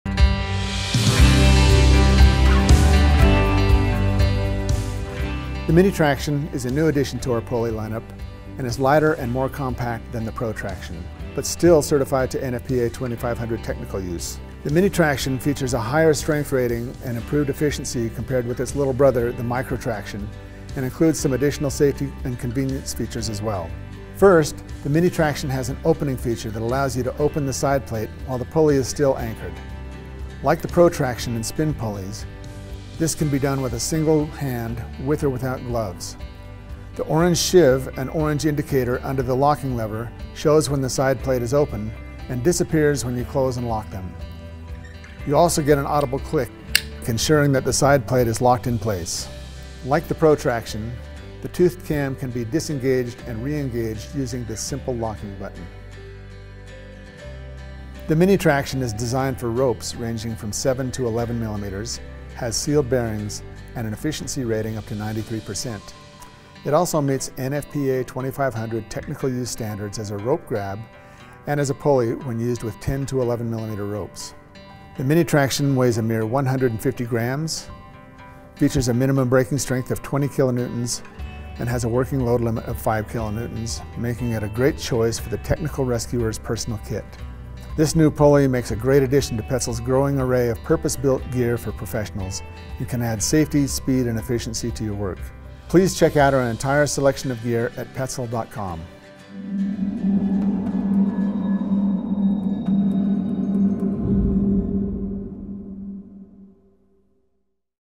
Petzl MINI TRAXION Overview
The MINI TRAXION progress capture pulley is designed for setting up haul systems. With a medium-diameter sheave and great efficiency, it is particularly suitable for hauling mid-weight loads. It is compact, with reduced weight and bulk, which is especially useful during technical rescue and rope access work.